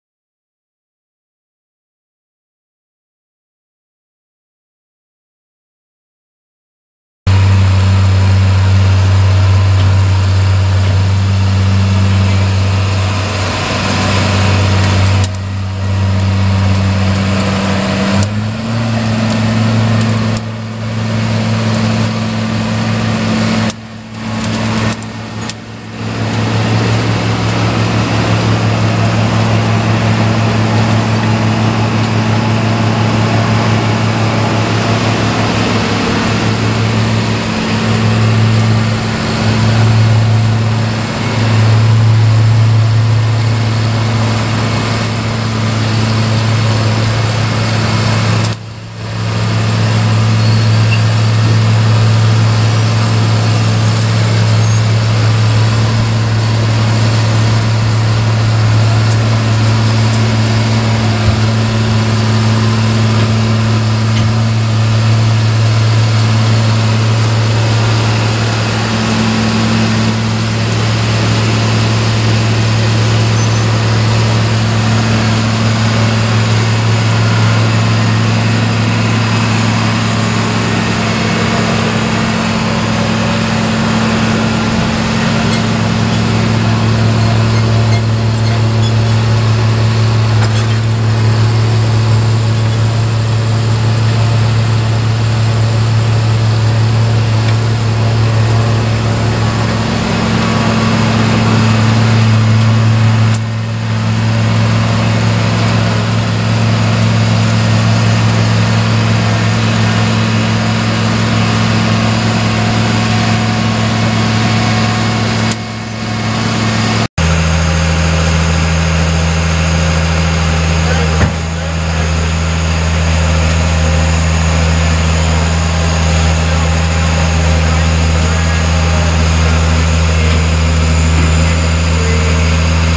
1991 Caterpillar AP1050 paver for sale | sold at auction May 11, 2012
1991 Caterpillar AP1050 paver
8,286 hours on meter
Caterpillar 3116 diesel engine
10&#039; VIP screed
10&#039; to 20&#039; width
Serial 2LF00121
Diesel screed heaters
Self adjusting 14" tracks
Dual controls
Recently replaced battery
Serial 1JC00081

It was located in Shawnee, KS, and we sold it to the highest bidder for $24,750.